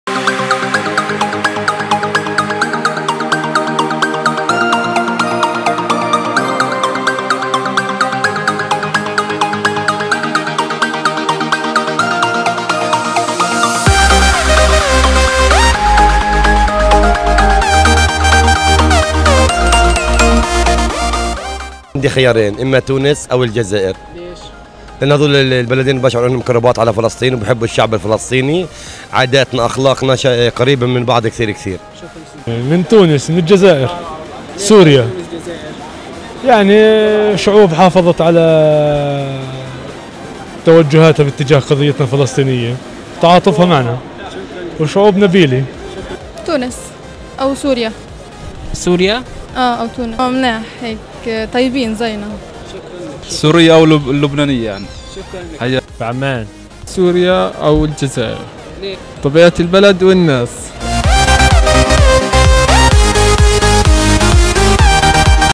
رام_الله: لو بدك تتزوج بنت مش فلسطينية.. من أي دولة عربية راح تختار؟!"
سؤال من شوارع رام الله "لو أتيحت لك فرصة الزواج بفتاة ليست فلسطينية.. فمن أي دولة عربية تختار؟!"
رام الله
الدول العربية
فلسطين
مصر
الجزائر